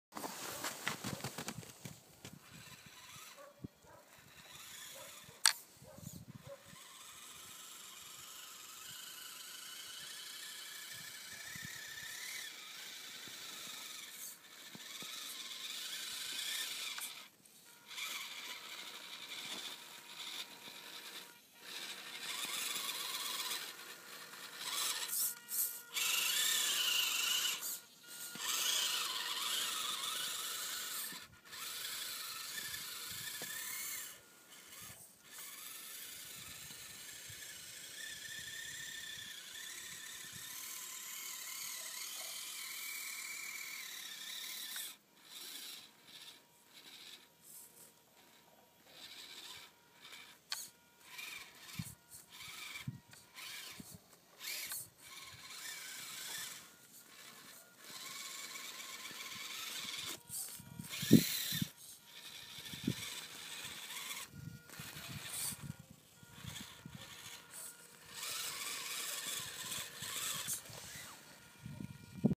Team losi micro rock crawler
Team losi micro rock crawler on snow in russia